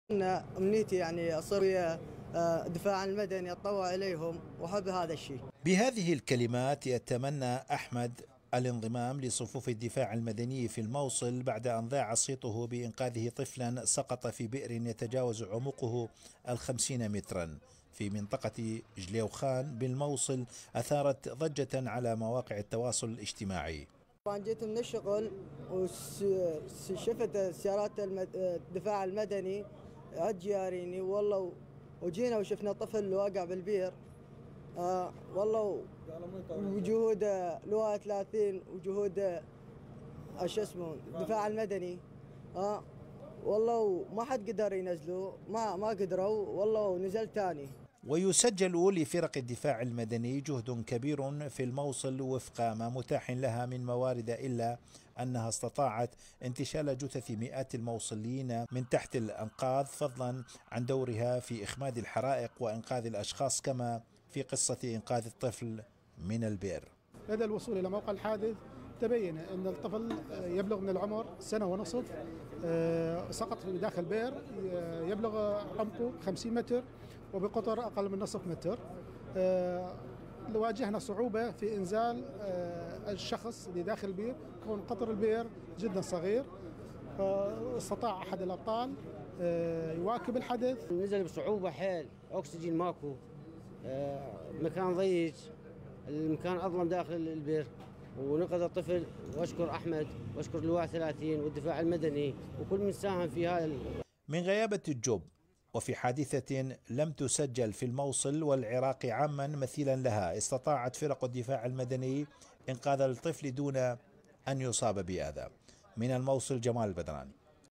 سبايدر مان الموصل .. احمد ينقذ طفلاً ويحلم بالالتحاق بالدفاع المدني
سبايدر مان الموصل ..
أحمد ينقذ طفلاً ويحلم بالالتحاق بالدفاع المدني